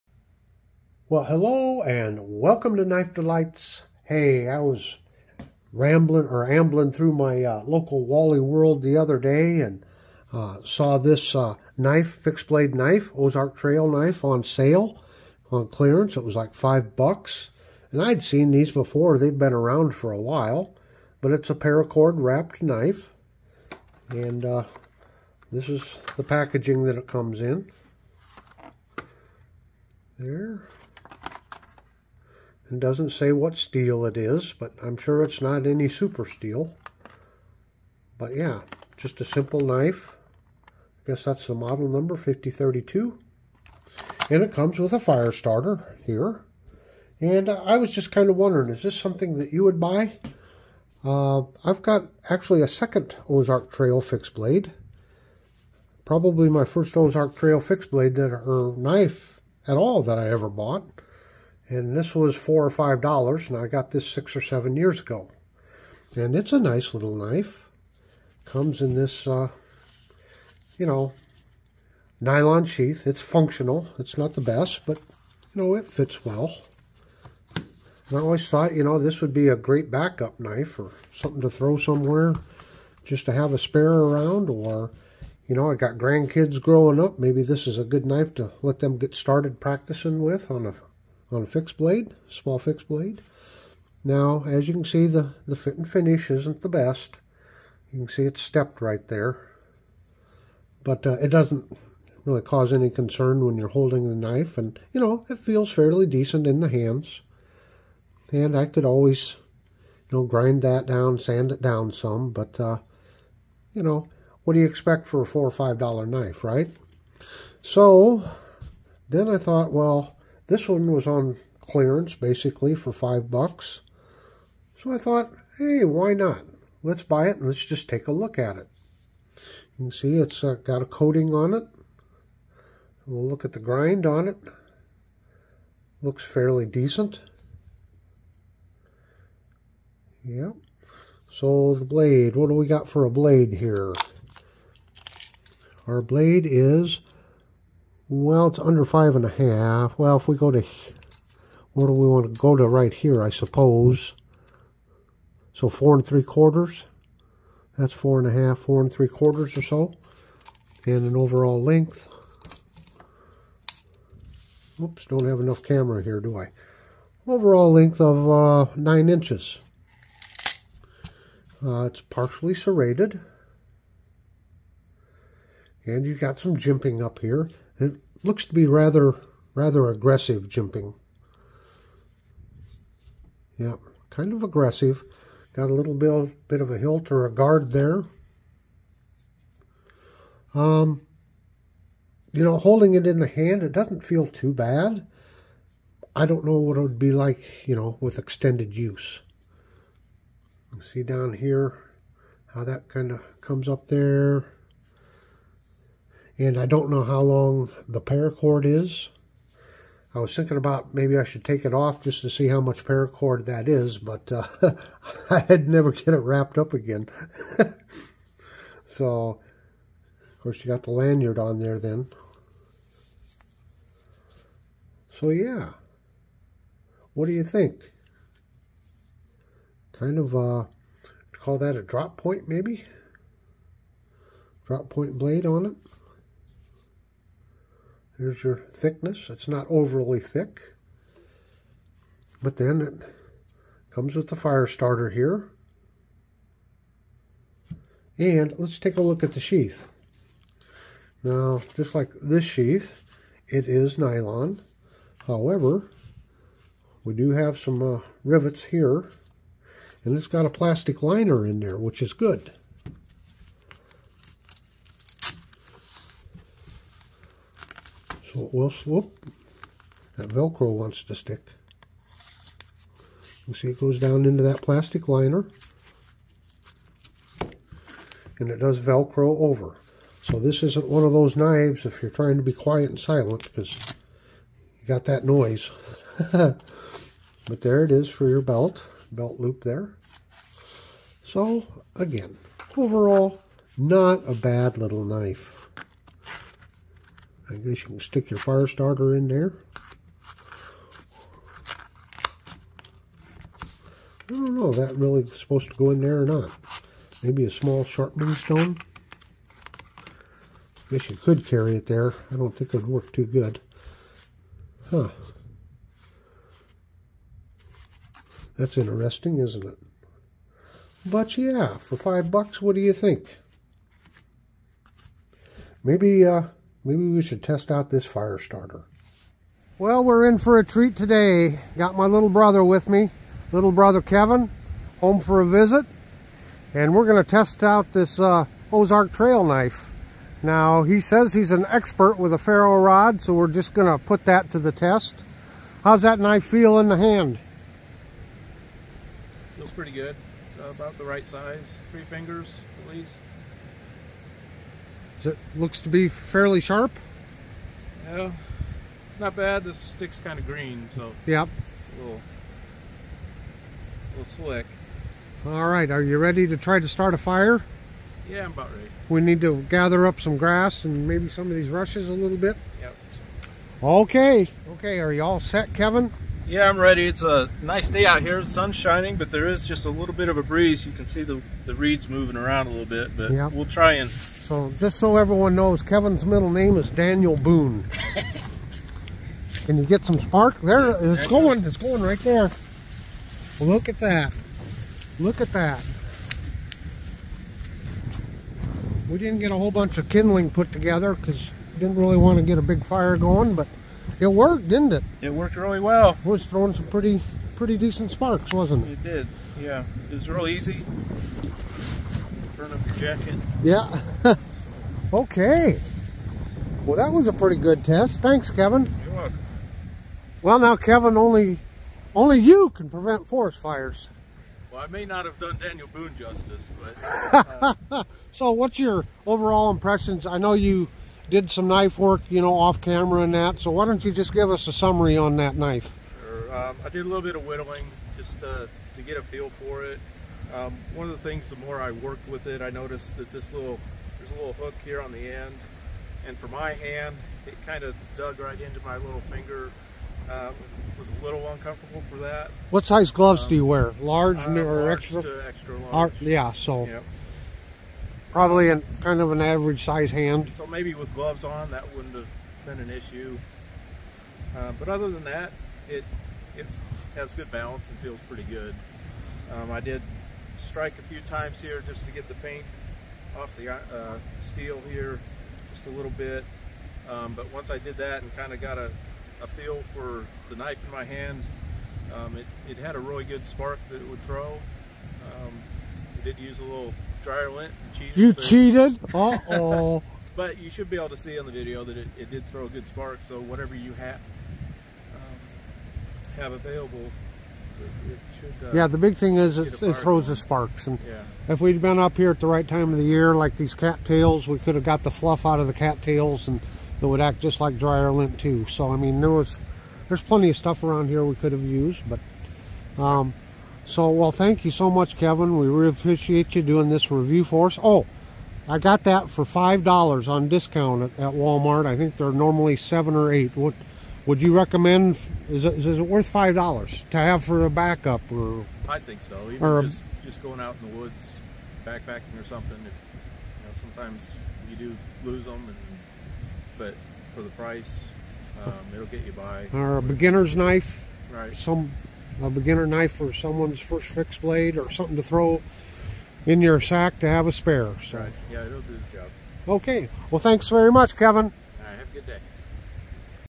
Testing The 5032 Ozark Trail Paracord Knife and Fire Starter
We begin will a tabletop review, then my little brother tests the fire starter.